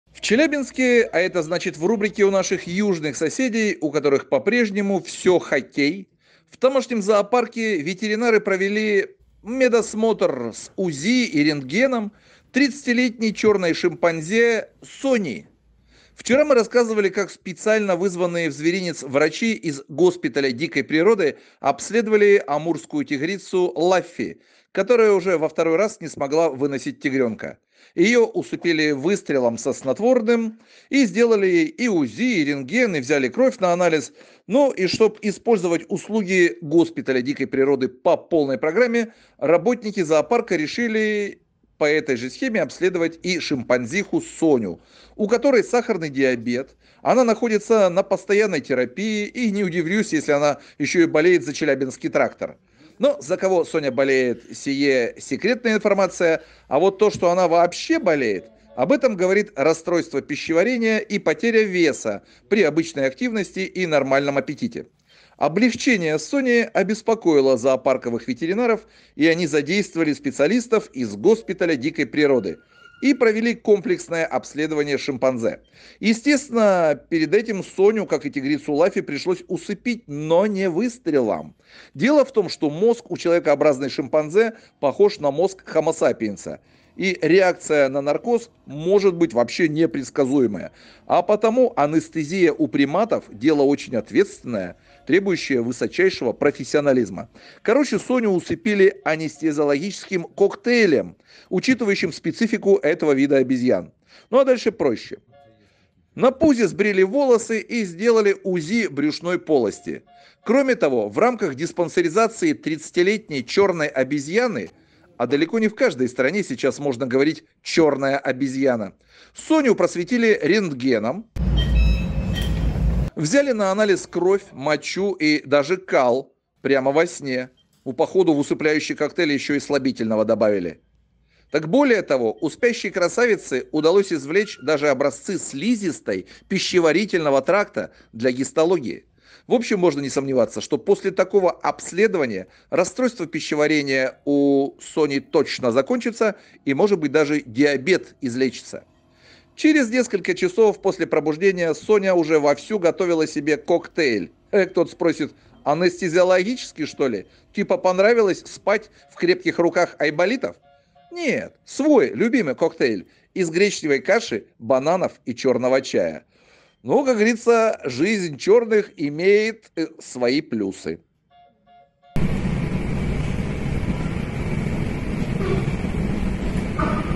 Чёрной обезьяне сделали УЗИ и рентген. Real video
Шимпанзе Соня стала худеть и её усыпили... коктейлем